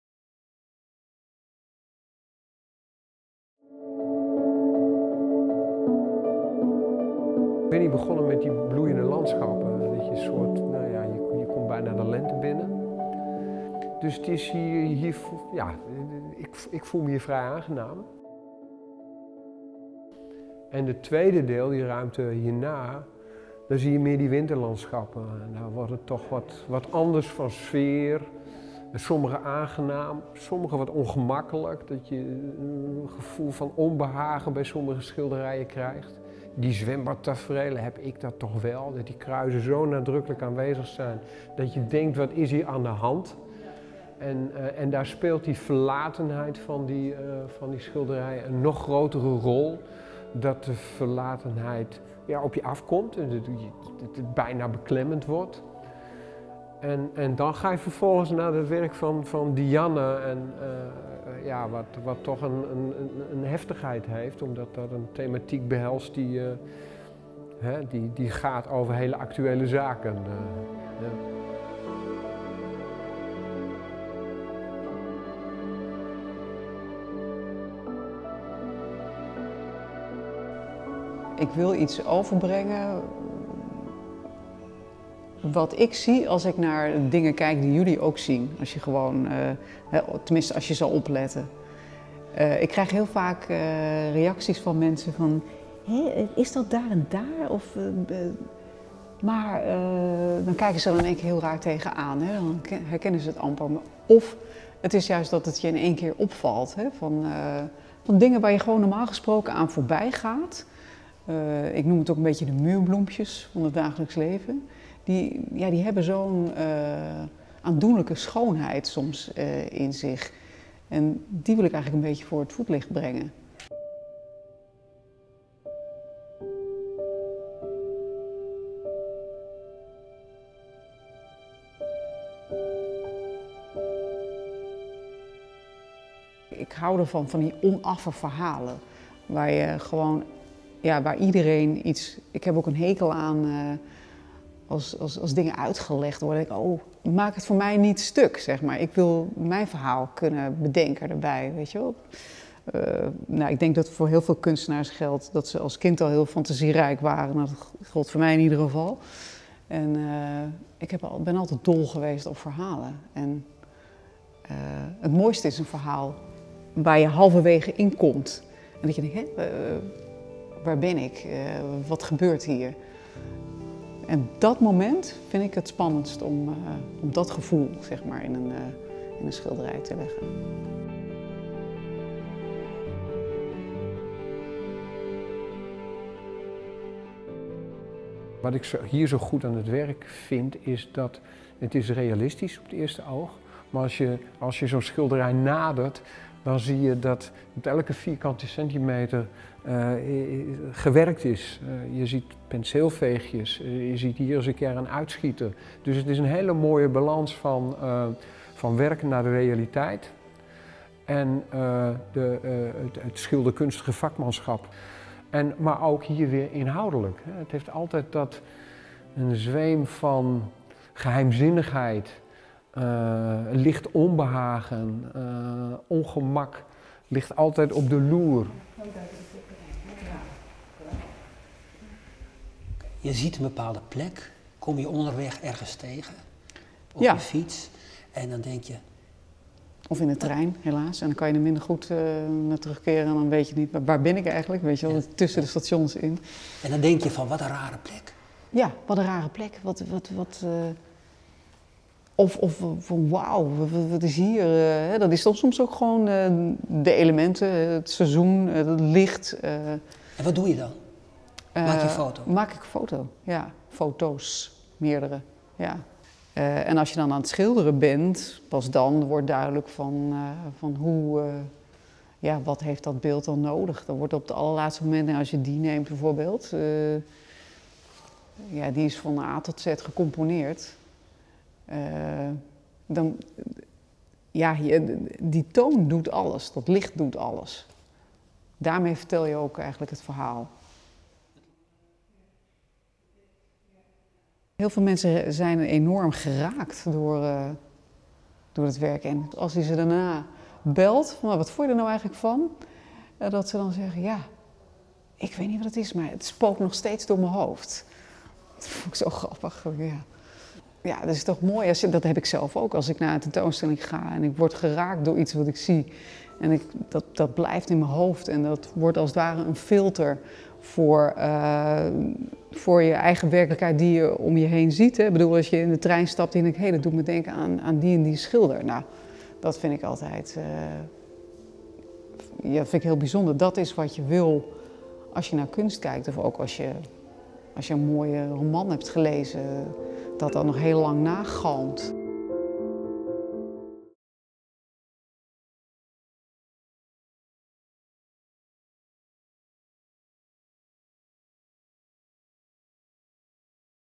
Landschappen aan de Buitenrand - Museum Belvédère
Korte versie van de film over en op de solotentoonstelling van Isabella Werkhoven ‘Landschappen aan de Buitenrand’ in Museum Belvédère, Heerenveen. Interview met Isabella Werkhoven en directeur-conservator Han Steenbruggen. Gemaakt door Albert Jensma en Jan Bensliman (Omrop Fryslan). De tentoonstelling is te zien van van 29 januari t/m 15 mei 2022. (De oorspronkelijke film op het YouTube-kanaal van Museum Belvédère begint met de exposant in het achterste deel van de grote zaal - Dianne Bakker. Dat deel is in deze versie weggelaten.)

(ENG) Short version - film with interview about and at the solo show Landscapes of the In-Between /Landschappen aan de Buitenrand at Museum Belvédère, Heerenveen Frisia. From 29 January  - 15 May 2022
Featuring director-curator Han Steenbruggen and Albert Jensma and Jan Bensliman (Omrop Fryslan - Frysian broadcaster). This is a shortened version of the original film at the Youtube channel of the museum. (The original video on the YouTube channel of Museum Belvédère starts with the exhibitor in the back of the large room - Dianne bakker. That part has been left out in this version.)